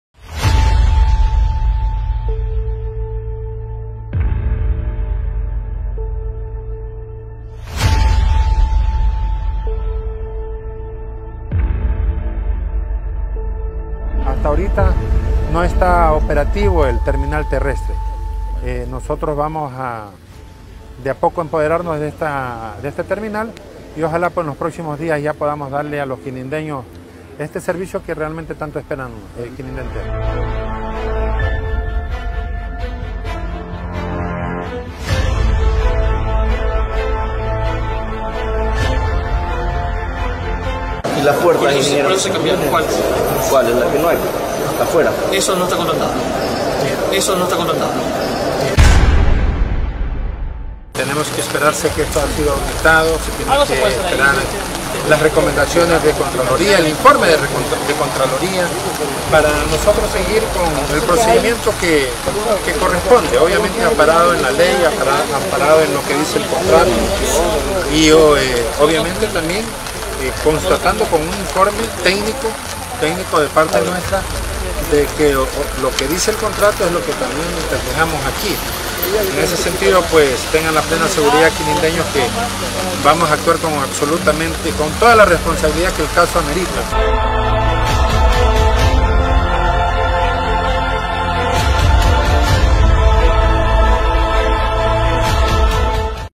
PROBLEMAS TERMINAL TERRESTRE DE QUININDÉ.
El recorrido en la Terminal Terrestre preocupó al Alcalde y Concejales por los problemas con los que se encontraron en una obra que se suponía estaba por inaugurarse.